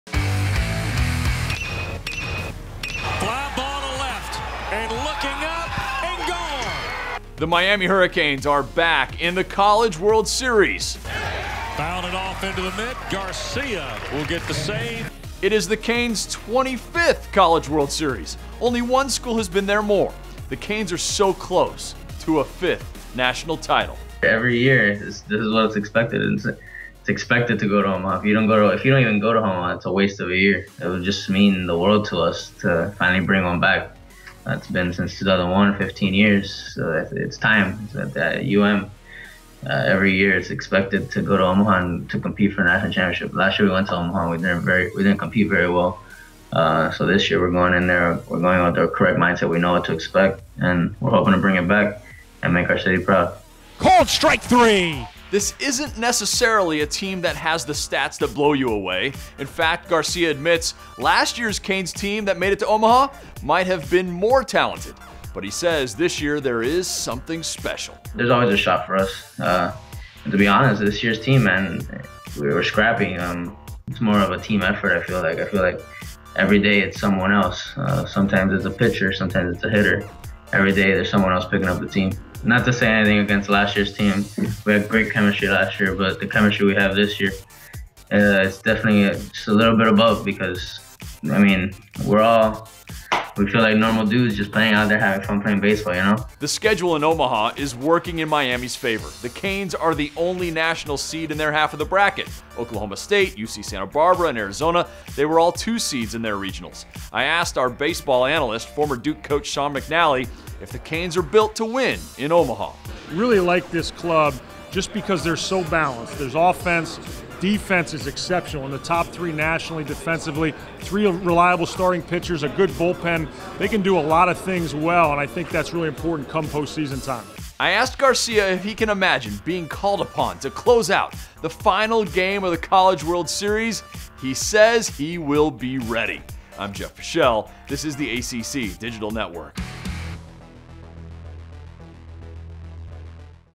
Miami Baseball Built To Win College World Series
Miami Baseball is back in Omaha for a second consecutive season.  This time around, the Hurricanes and their closer, Bryan Garcia plan to play their way into college baseball lore.  Garcia tells Jeff Fischel of the ACC Digital Network how amazing it would be to take the title in Omaha, which begins on June 18 at 8 PM against Arizona.